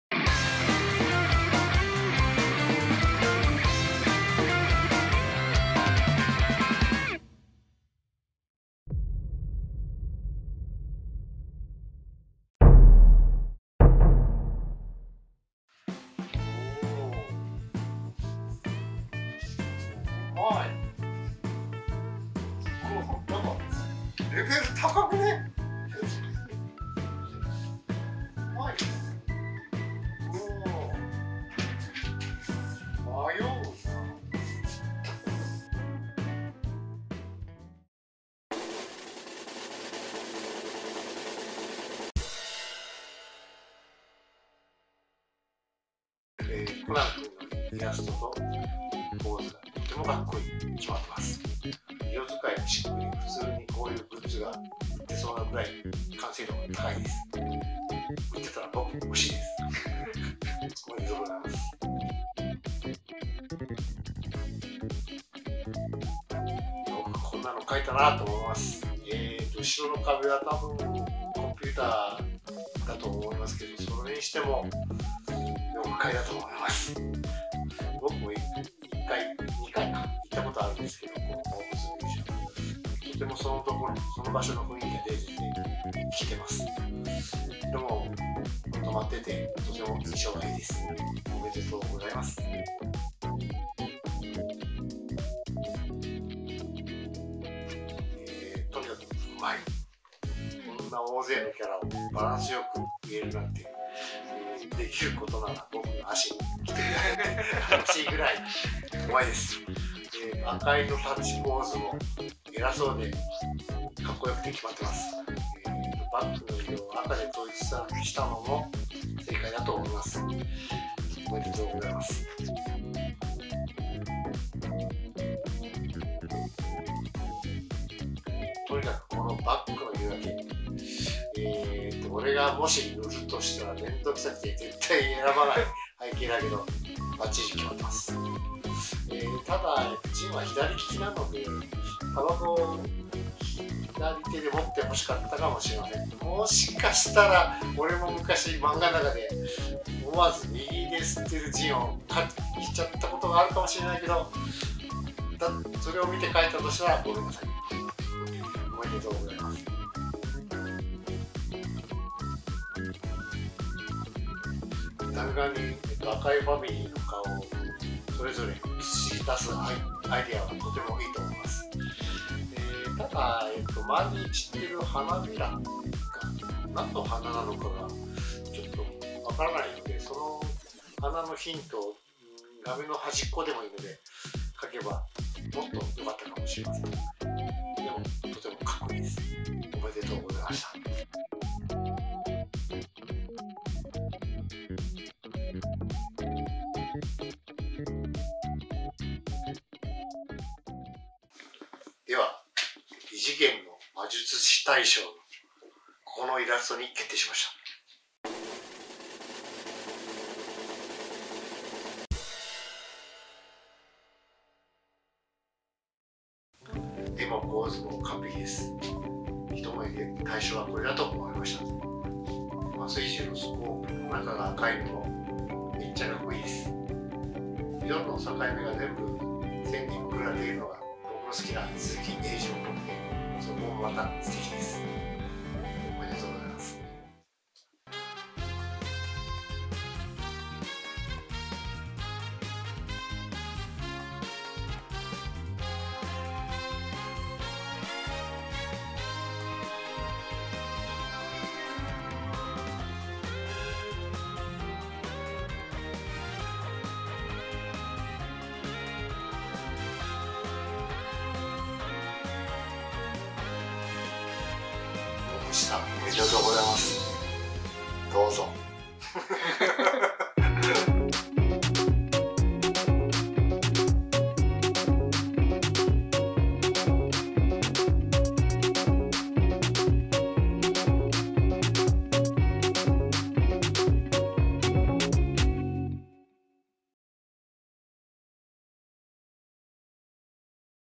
劇場版「名探偵コナン」RELOAD PROJECT［異次元の魔術師］イラストコンテスト結果発表！！【サンデー文化祭ONLINE2021】
イラストコミュニケーションサービス「pixiv」にて開催した劇場版『名探偵コナン 緋色の弾丸」公開記念イラストコンテスト［異次元の魔術師］の選考結果を、受賞作品への青山剛昌先生によるコメントと共に発表します！沢山のご応募ありがとうございました！

●週刊少年サンデーが送るオンラインイベント【サンデー文化祭ONLINE2021】開催中！
3月26日（金）～4月23日（金）
『サンデー文化祭ONLINE2021』特設サイトはコチラ

劇場版『名探偵コナン 緋色の弾丸』
2021年4月16日（金）より全国東宝系にてロードショー！